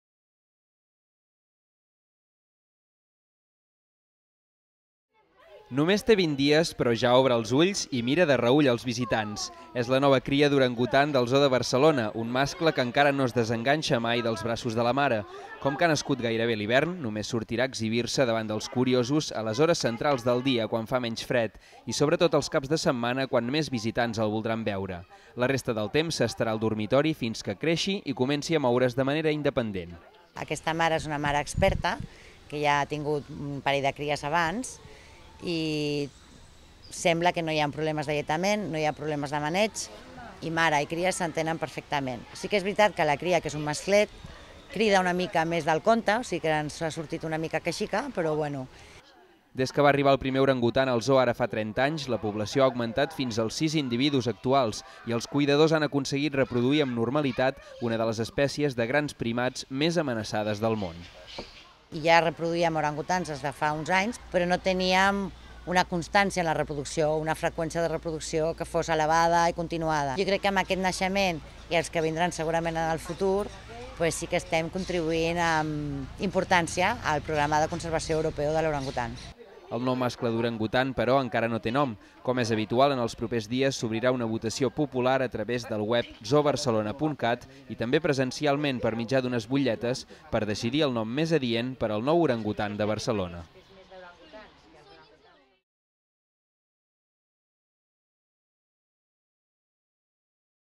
Un nou orangutan al Zoo
La nova cria té només 20 dies i no se separa de la mare. És el 6è exemplar d'una espècie que els cuidadors han aconseguit reproduir amb normalitat.